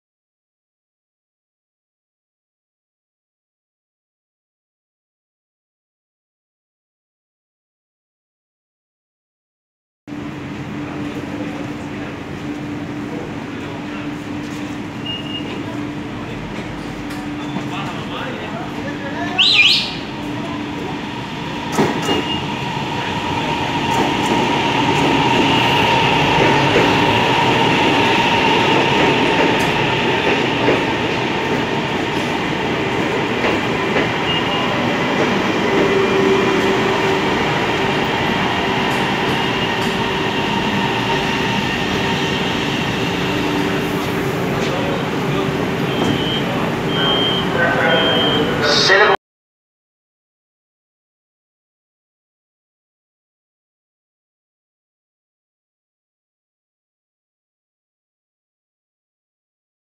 Línea Roca. Formación CSR arribando a Ezeiza.
Línea Roca. TRENES ARGENTINOS. En este video podrá observarse el arribo a dicha terminal del servicio eléctrico proveniente de Plaza Constitución.
Recuerden que para contenido extra, fotos, novedades y mucho más, el canal tiene INSTAGRAM
Gracias por serguirme!!!